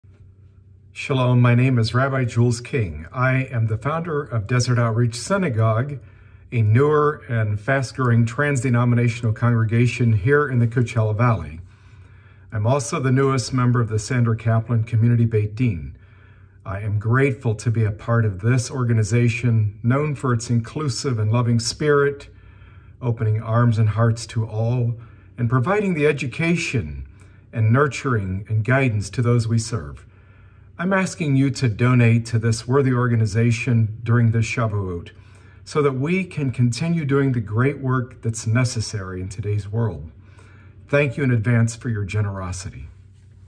Rabbi Jules King asks for your support for the Sandra Caplan Community Bet Din.
Rabbi King is the newest member of the Sandra Caplan Community Bet Din. He is founder of Desert Outreach Synagogue in the Coachella Valley.